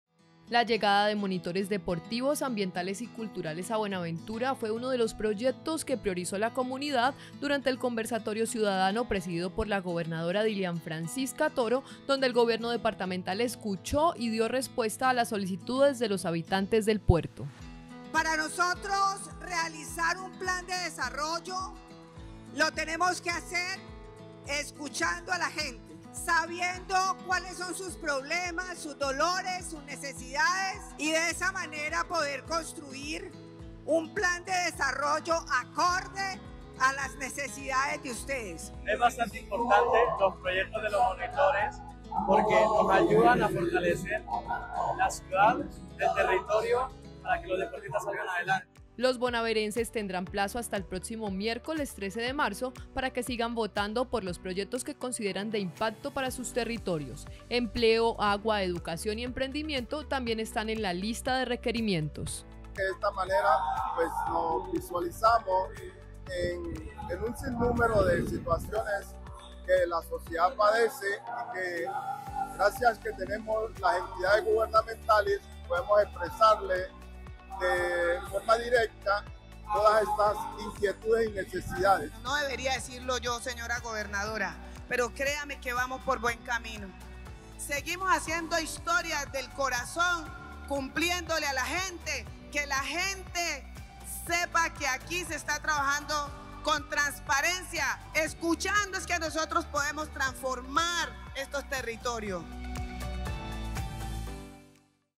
Bonaverenses priorizaron proyectos para el desarrollo de sus territorios
La llegada de monitores deportivos, ambientales y culturales a Buenaventura, fue uno de los proyectos que priorizó la comunidad durante el Conversatorio Ciudadano presidido por la gobernadora Dilian Franscisca Toro, donde el gobierno departamental escuchó y dio respuesta a las solicitudes de los habitantes del Puerto. Hasta el miércoles 13 de marzo hay plazo para votar.